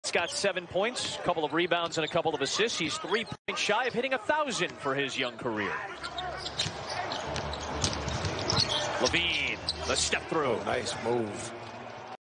Ball Screens- Euro- Zach LaVine
Step Up Angled Ball Screen
Fast and Decisive to the screen
Slow off the screen
Euro thru contact

Get Your Film Reps Up! Study the Skills.

What keys and details do you see?